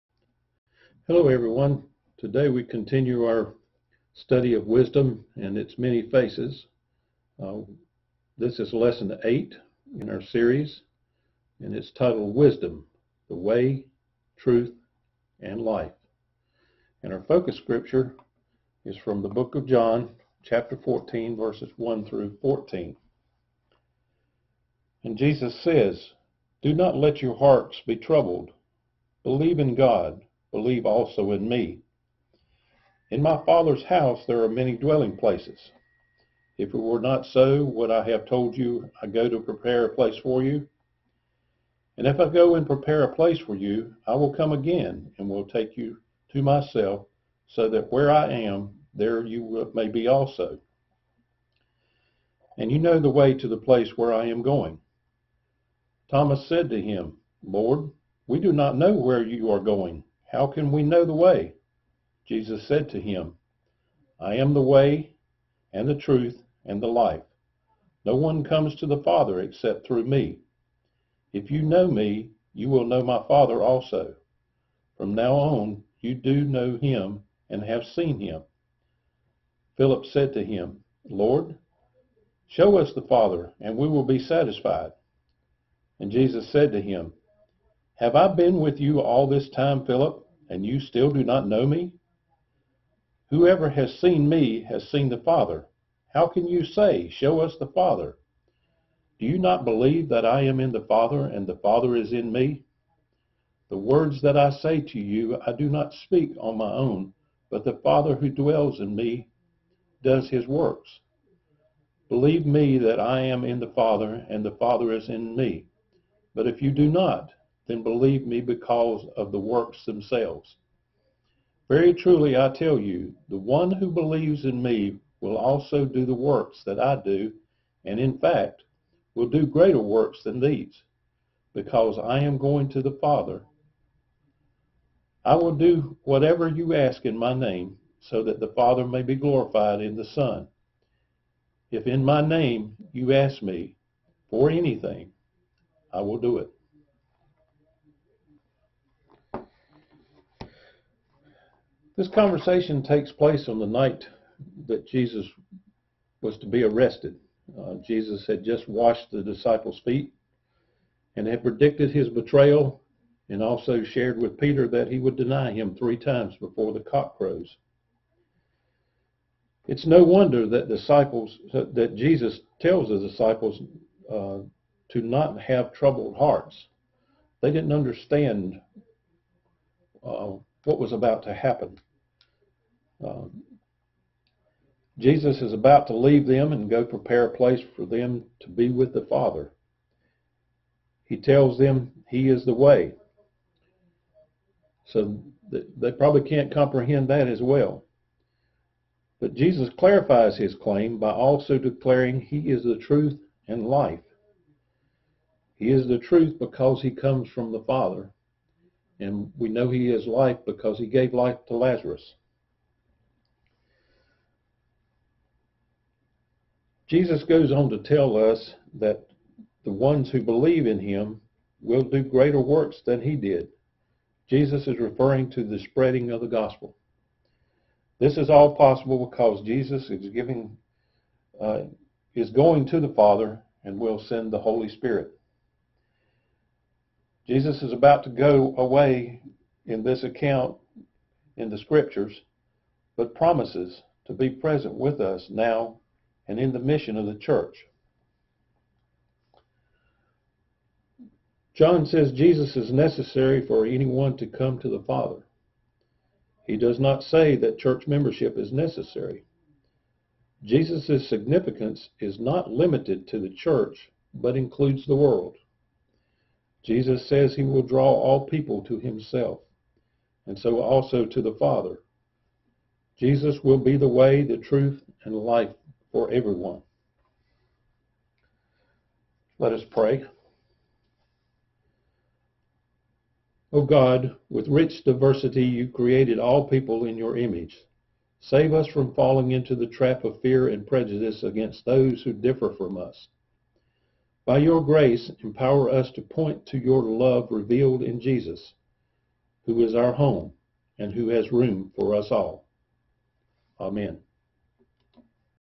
SS19 The Way Truth and Life
Steve leads this session "The Way Truth and Life" in the series the Many Faces of Wisdom.   Explore how Jesus is the Way, and some of what that might mean.